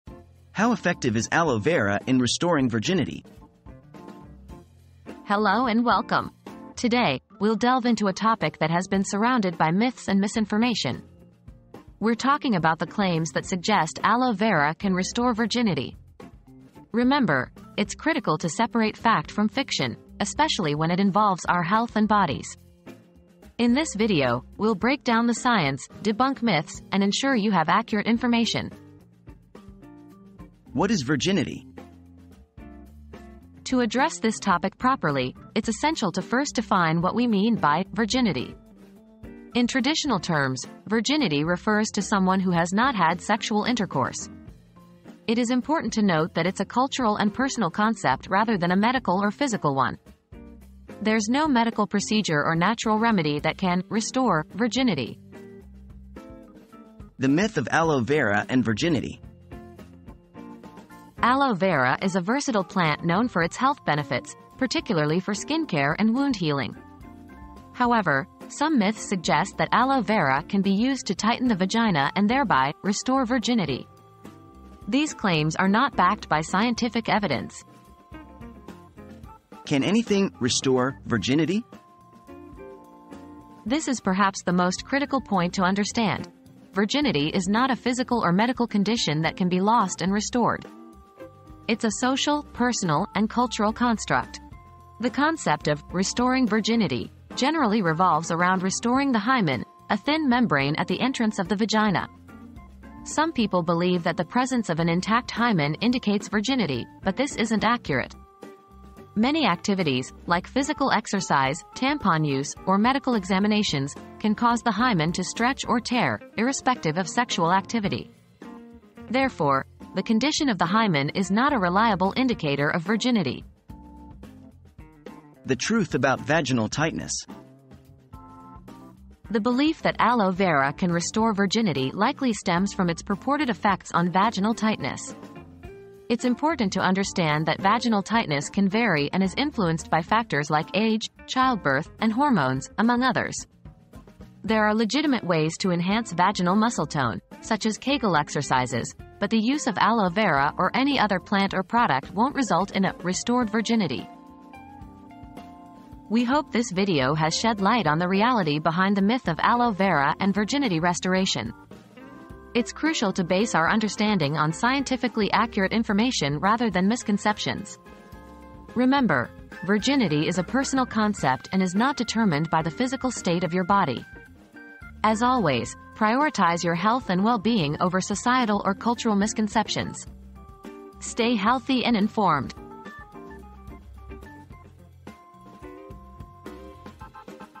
How effective is aloe vera in restoring virginity?
Debunking Myths: Aloe Vera and Virginity • Uncover the truth about the myths surrounding aloe vera and virginity restoration in this informative video. Join us as we break down the science, debunk misconceptions, and provide accurate information on this controversial topic. Learn why virginity is a cultural and personal concept, not a medical condition, and discover why claims about aloe vera's ability to restore virginity lack scientific evidence. Don't miss out on this eye-opening discussion that separates fact from fiction!

00:00 • How effective is aloe vera in restoring virginity?
00:30 • What Is Virginity?
00:57 • The Myth of Aloe Vera and Virginity
01:19 • Can Anything 'Restore' Virginity?
02:05 • The Truth About Vaginal Tightness